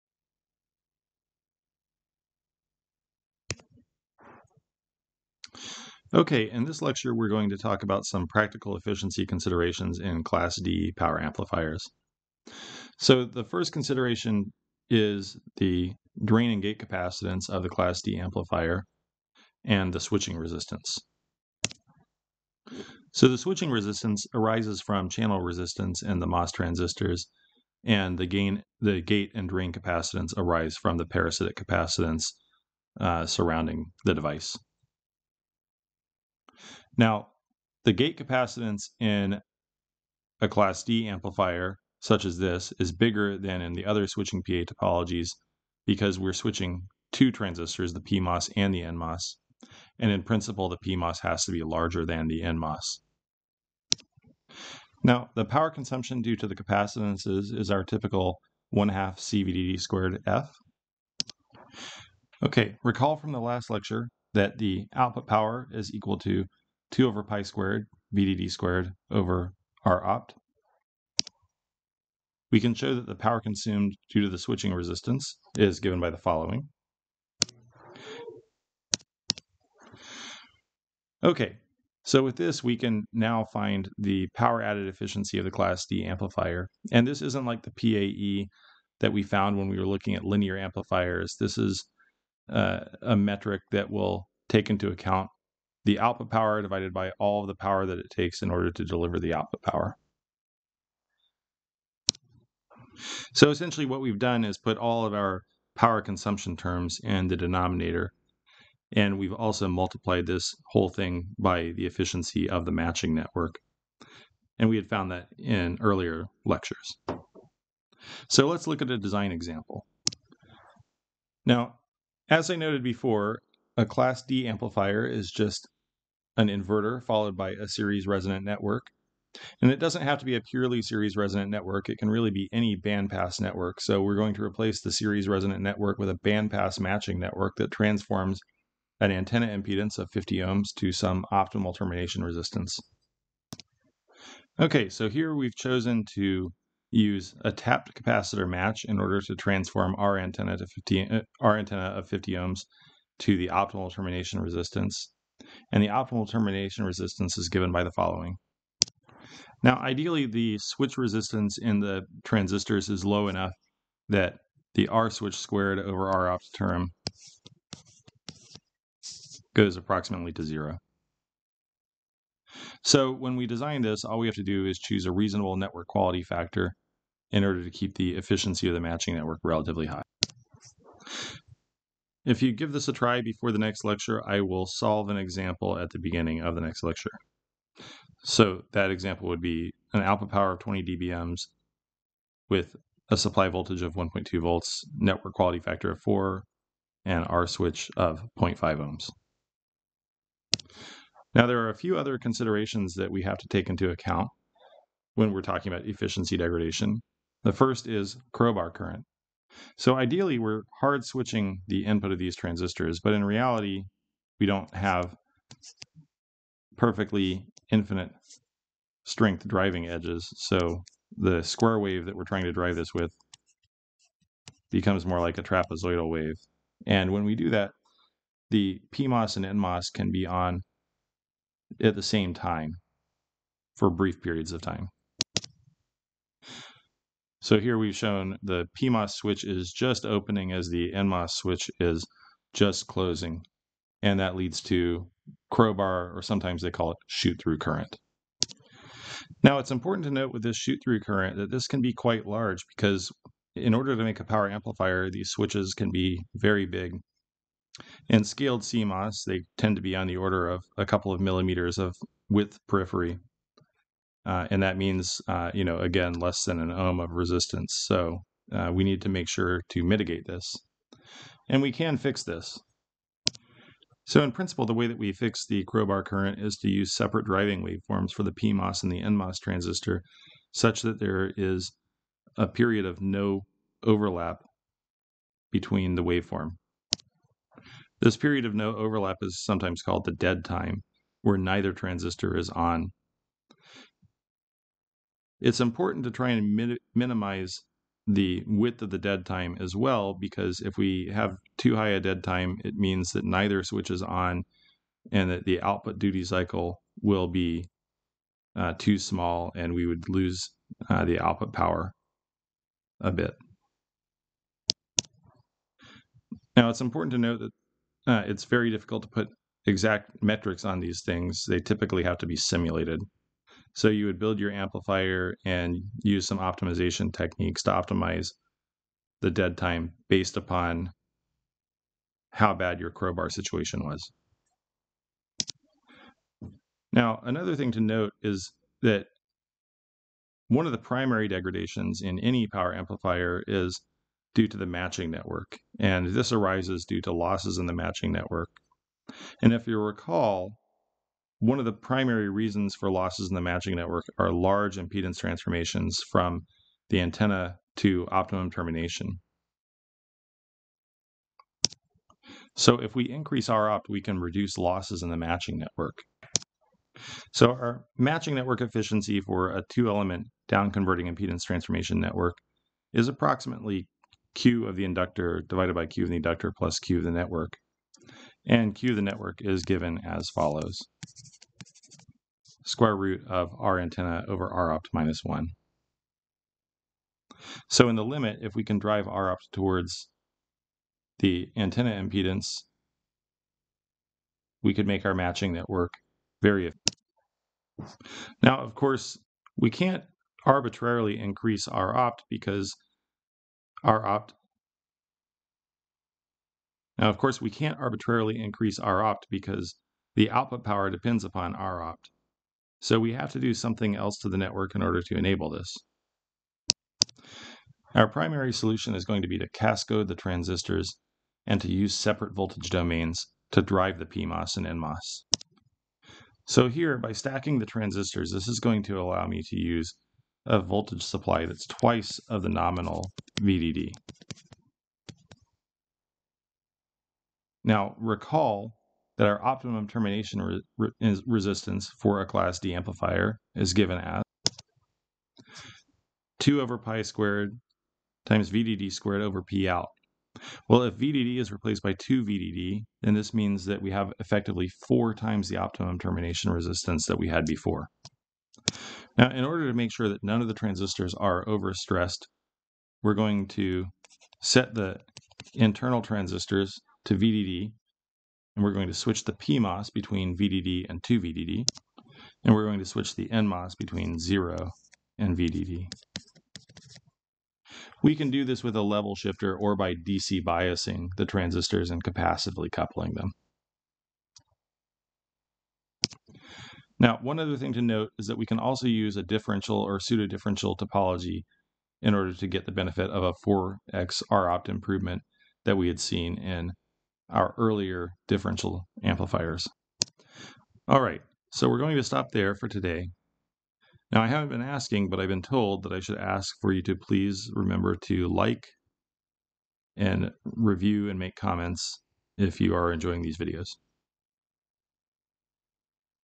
Lecture 17.2 - Losses in Class-D Power Amplifiers
In this video, we examine losses in the class-D power amplifier due to switching resistance, parasitic capacitance and crowbar current, and examine the benefit of cascoding.